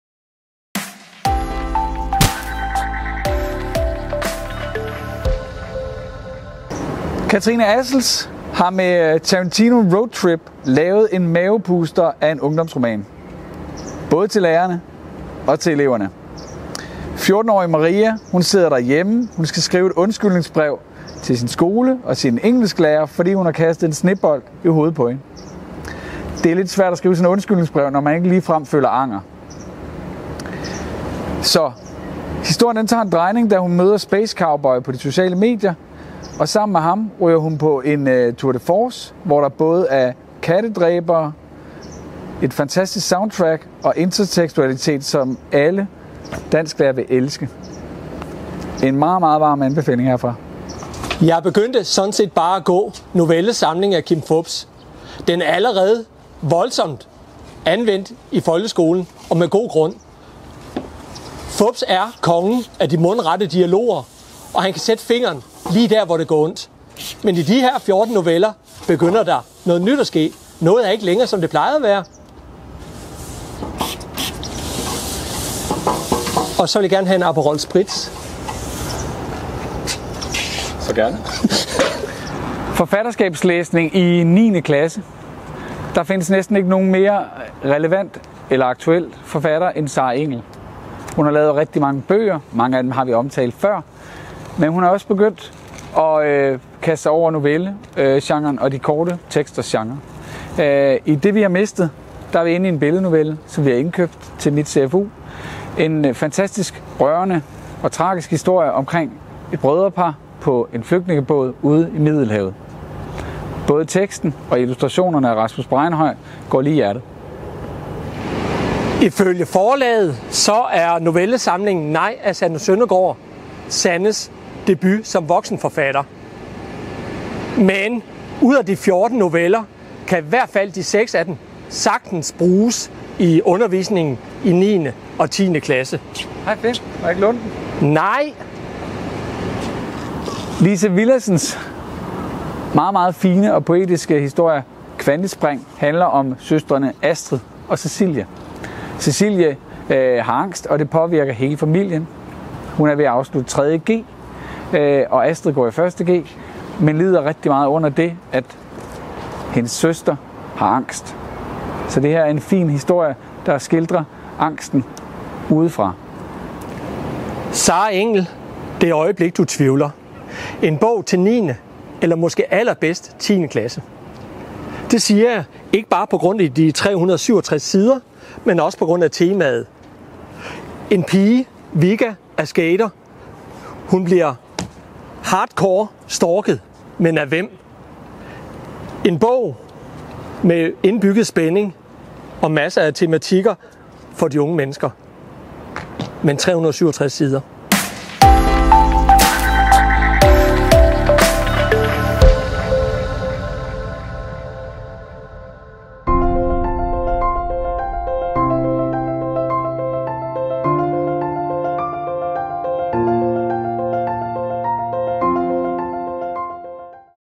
Fra 9. og 10. kl. - Nye indkøb og vejledninger til dansk i udskolingen
Jan og Finn præsenterer nye bøger og materialer til dansk i udskolingen. eller læs mere her